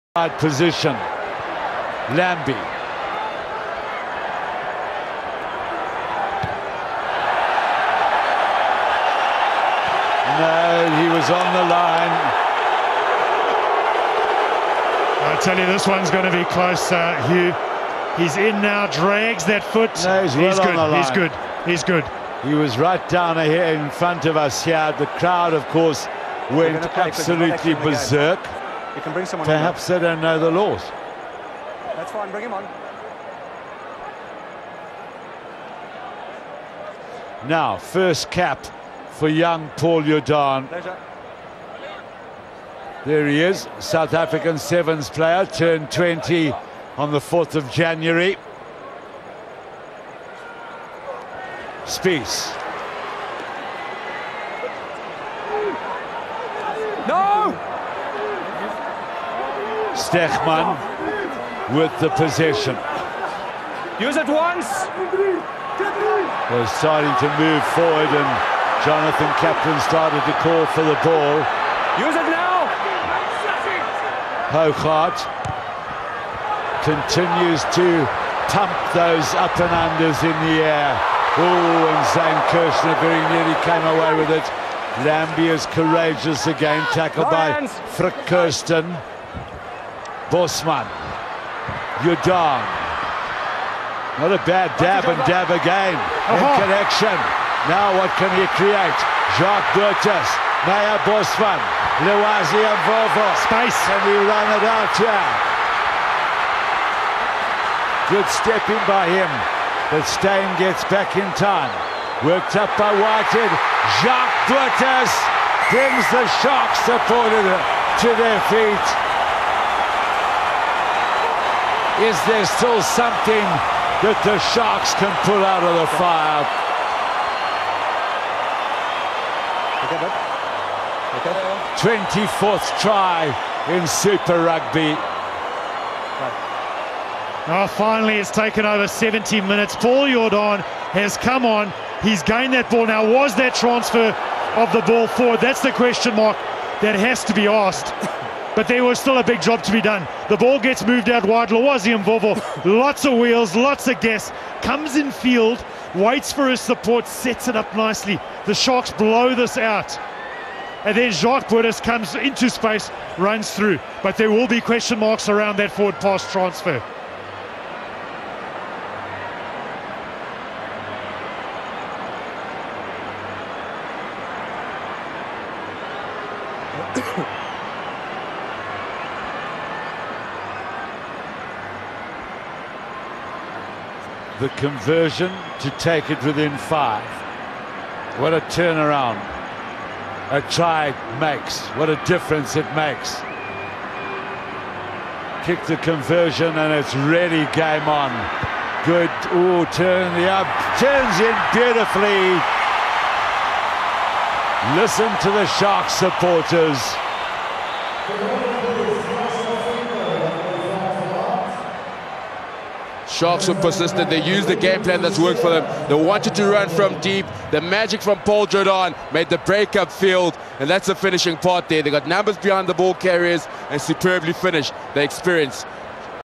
Sharks debut Paul Jordaan
20-year old Paul Jordaan makes his debut for the Sharks and immediately shows his skills by preparing the first try of the match!